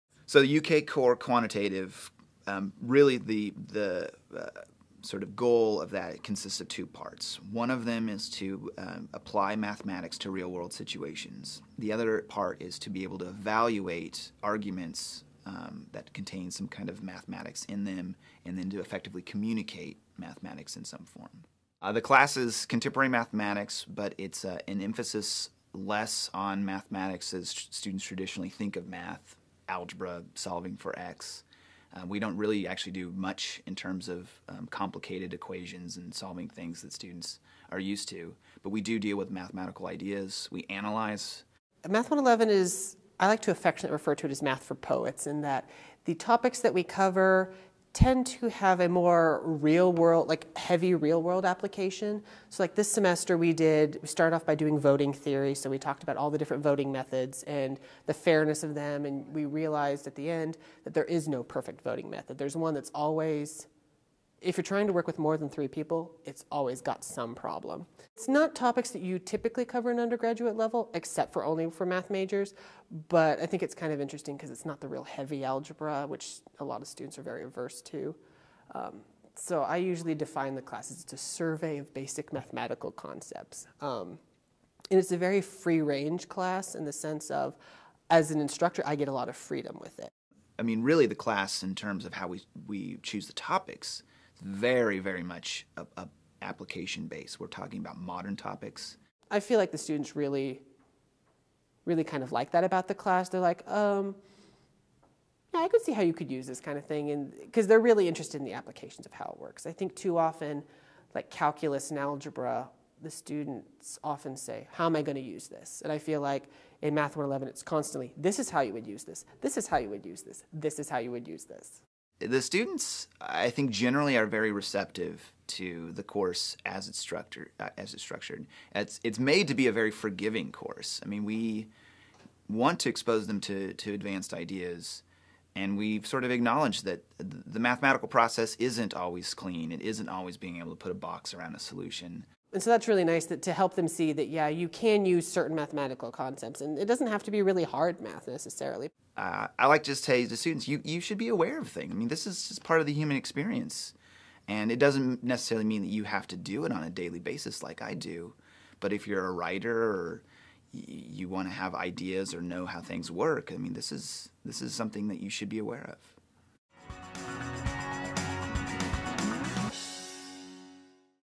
UK 'Contemporary Math' Course Has Modern Applications
A new University of Kentucky Core class sets out to prove that in fact you can use advanced mathematics applications to solve real world problems. To demonstrate how math can be used to examine almost any question, faculty teaching MA 111 show how math can be used to clarify and critically evaluate issues related to coding, graphing, compensation and even voting.